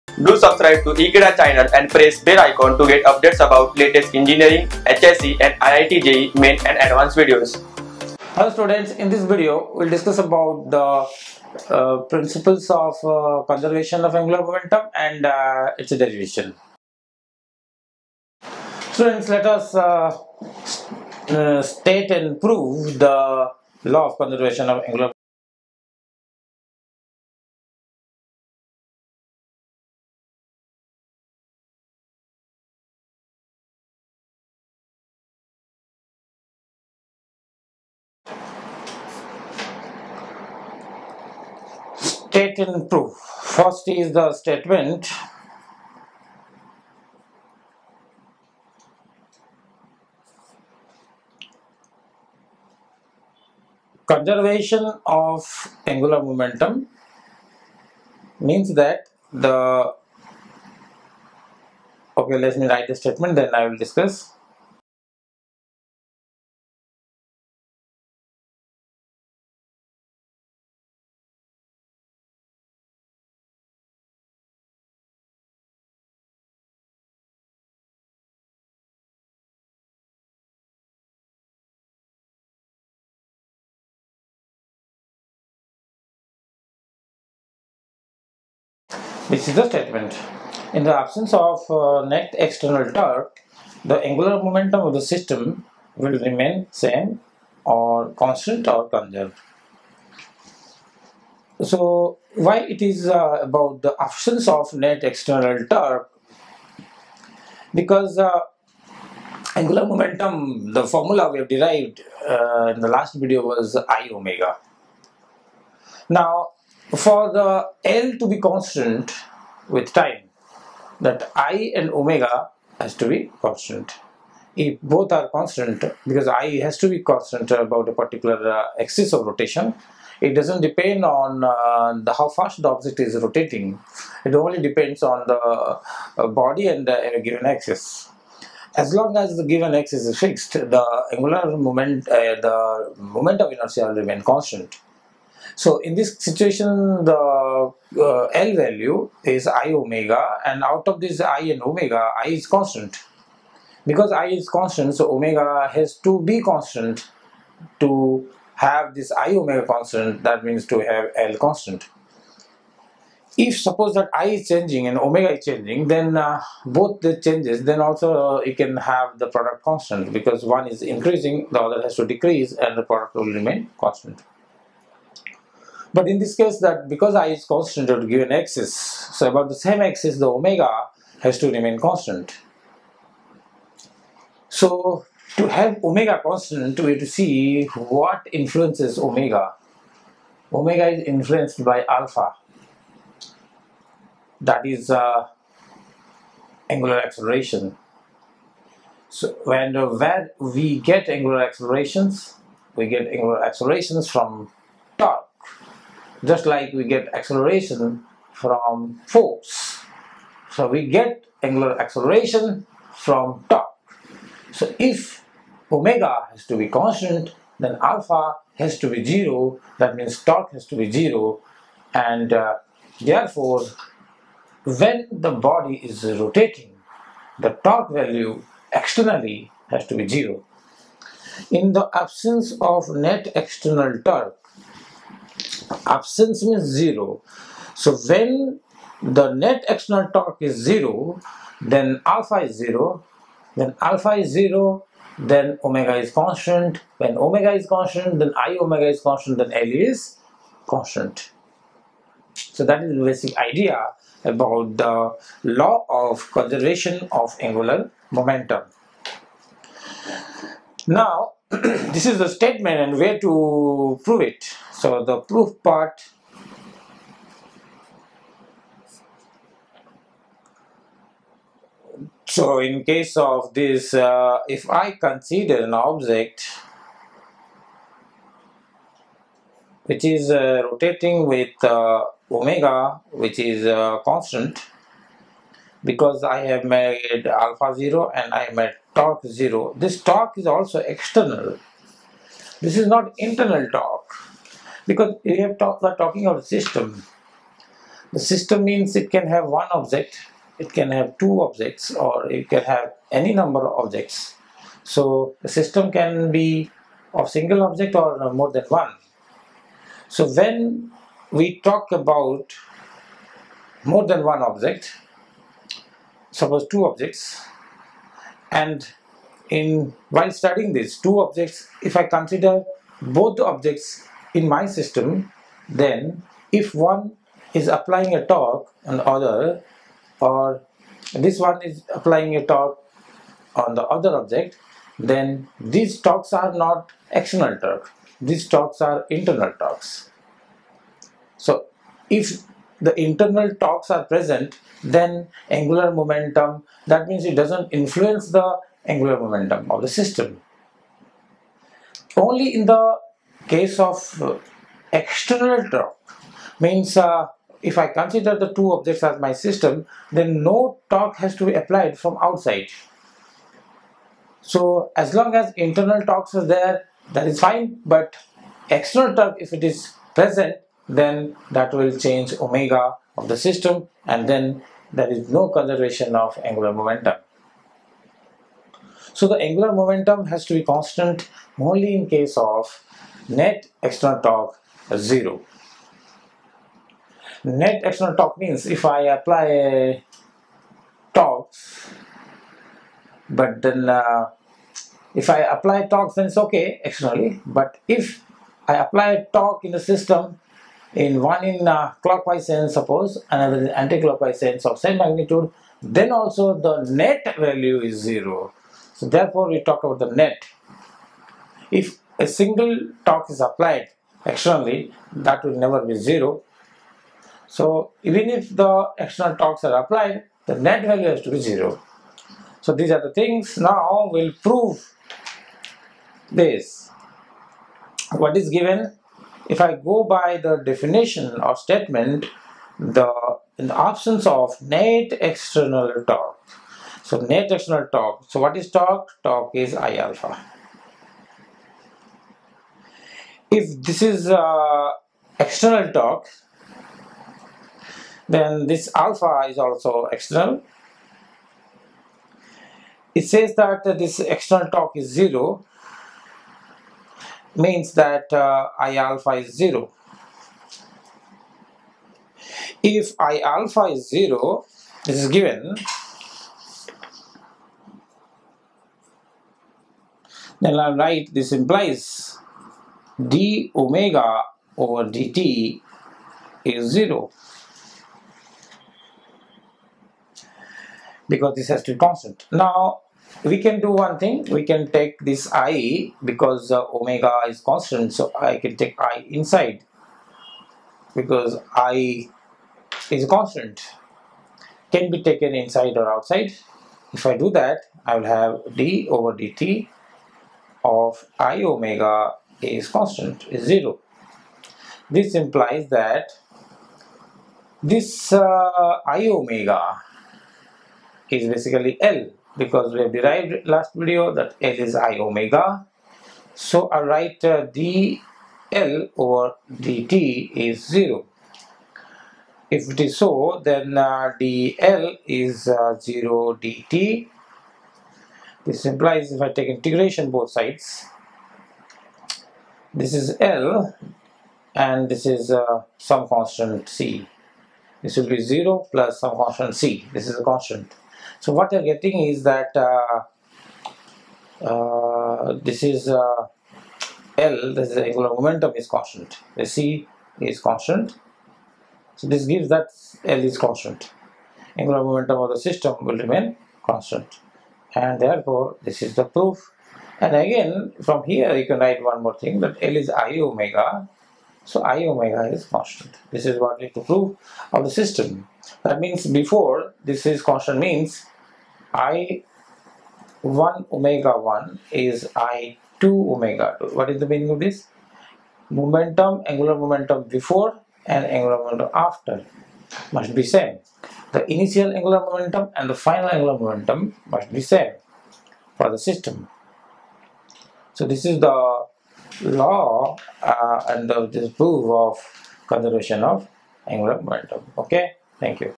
Law of Conservation of Angular Momentum - Rotational Motion - Physics Class 12
Law of Conservation of Angular Momentum Video Lecture from Rotational Motion chapter of Physics Class 12 for HSC, IIT JEE, CBSE & NEET.

Happy Learning : )